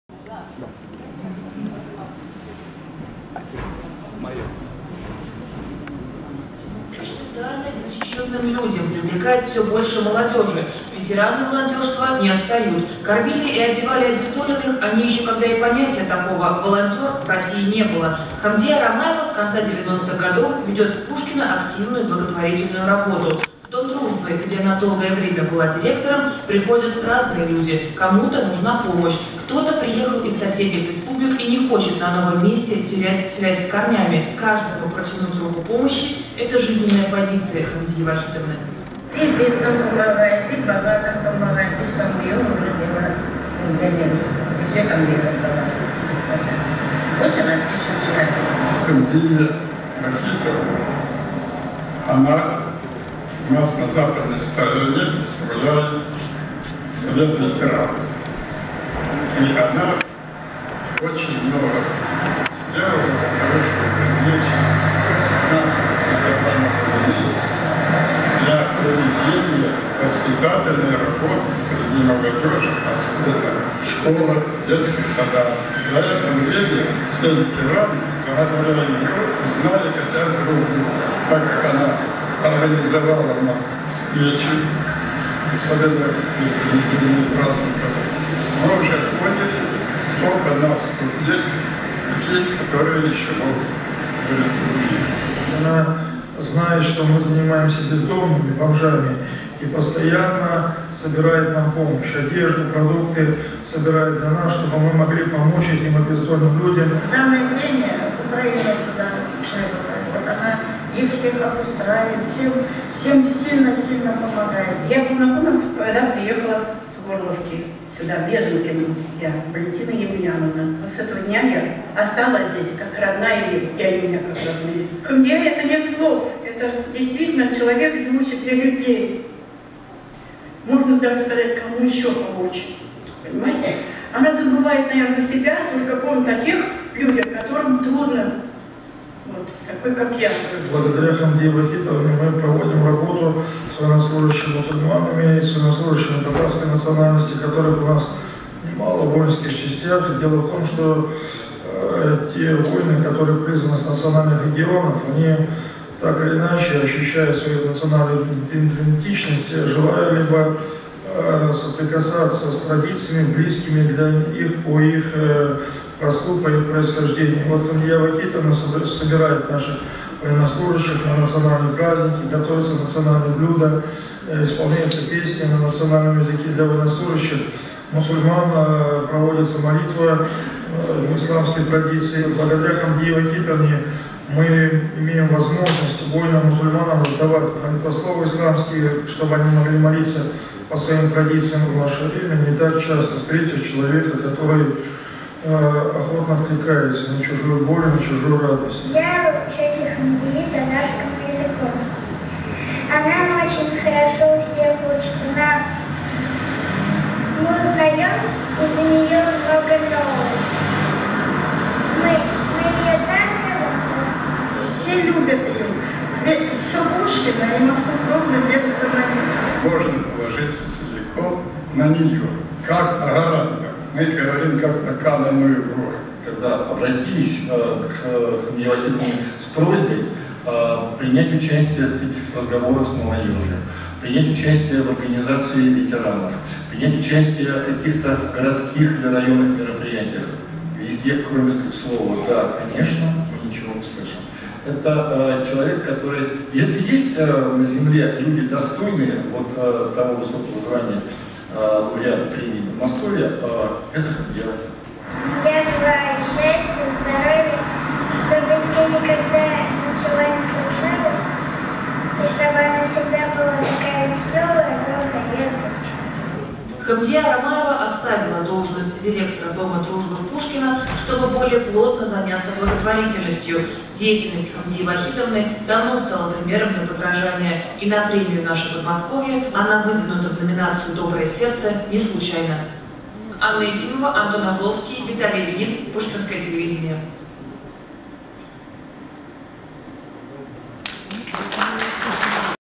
1. Фильм о Хамдие Рамаевой
13 октября скончалась одна из родоначальниц татарского движения в Московской области Хамдия Вахитовна Рамаева
Сегодня ночью после тяжёлой болезни ушёл из жизни человек, с именем которого связано официальное становление татарского движения в Подмосковье.
 Хамдия Вахитовна родилась в многодетной татарской семье в селе Старое Зелёное Старокулаткинского района Ульяновской области. Там же закончила татарскую школу, по окончании которой поступила в педагогический институт на филологический факультет. Татарский и русский язык стали для неё родными языками. На вопрос, на каких языках Хамдия Вахитовна мыслит, она отвечала: на русском и на татарском. Хамдия Вахитовна преподавала русский язык и литературу в Татарстане и Ульяновской области. С 1980 года до выхода на пенсию в 1999 году - учила детей в Майской средней школе в Софрино-1 Пушкинского района Московской области - её выпускники до её последних дней поддерживали с ней самые тёплые и доверительные отношения.
     Хамдия Вахитовна всегда активно занималась общественной работой, стояла у истоков создания в Пушкинском муниципальном районе Интернационального Дома Дружбы. В 1997 году создала татарское общество в городе Пушкино, и уже в октябре 2001 года вместе со своими единомышленниками зарегистрировала Местную татарскую национально-культурную автономию “Вазыфа”, ставшую одной из лучших в структуре Региональной татарской национально-культурной автономии Московской области, которую тогда возглавлял Ильшат Мирзагитович Сафаргалиев. Главной задачей этой общественной организации стало сохранение татарских традиций, поддержка земляков и помощь нуждающимся. Именно поэтому было выбрано такое название, в переводе на русский язык означающее «Долг».
     Много начинаний и интересных дел родилось в Пушкинском районе при непосредственном личном участии Хамдии Вахитовны. Возглавляя МТНКА и одновременно являясь одним из учредителей Местной религиозной организации мусульман г.Пушкино и Пушкинского района, она преподавала татарский и арабский языки, многое делала для укрепления межнациональных и межконфессиональных отношений.
     С 2003 года в Пушкинском районе начали проводится татарские национальные праздники “Сабантуй”, различные благотворительные акции по поддержке ветеранов, воспитанников детских домов, жителей Донецкой и Луганской республик, посещения воинских частей, где проходят срочную службу солдаты-мусульмане, многочисленные мероприятия по поддержке и популяризации татарской культуры, традиций и языка среди жителей Пушкинского района. Хамдия Вахитовна сумела объединить вокруг себя неравнодушных и инициативных людей, организовывала коллективные экскурсии и поездки в Москву, Казань, другие города Московской области для участия в Сабантуях, посещения концертов и спектаклей татарских исполнителей, принимая при этом большое личное участие. Татары Пушкинского района благодарны Хамдие Вахитовне за организованные поездки на фестивали “Музыкальное искусство татарского народа - Подмосковью”, на первую в России выставку “Татарские исполнители на пластинках Апрелевского завода грампластинок”, в Московскую среднюю общеобразовательную школу с этнокультурным татарским компонентом образования № 1186, в мечети города Москвы и др.
     Х.В. Рамаева - один из самых ярких авторов очерков и статей в выходившей в первые годы существования РТНКА МО газеты "Ватандашлар" ("Мои соотечественники").
     Хамдия Вахитовна Рамаева - ветеран труда. Имеет звание “Почётный ветеран Подмосковья”. Возглавляла первичку Совета ветеранов на западной стороне Пушкино, была членом Общественной палаты Пушкинского района.
     Хамдия Вахитовна стала первым в Московской области представителем мусульманских национально-культурных автономий, удостоенным медалью Совета муфтиев России «За духовное единение». Медаль эта вручается за большой вклад в дело распространения национальных культурных и духовных традиций, за многолетнюю плодотворную работу, проявленные личное трудолюбие и высокую гражданскую активность. Хамдия Вахитовна имеет много наград. Она является лауратом премии Губернатора Московской области “Наше Подмосковье” 2014 года.
     Хамдию Вахитовну отличали доброжелательность, коммуникабельность, искренность, честность, готовность прийти на помощь в трудную минуту. Ушла из жизни активный, целеустремленный, жизнерадостный, внимательный, очень добрый и светлый человек, неустанно заботившийся, даже в дни тяжелой болезни, о будущем татарского народа.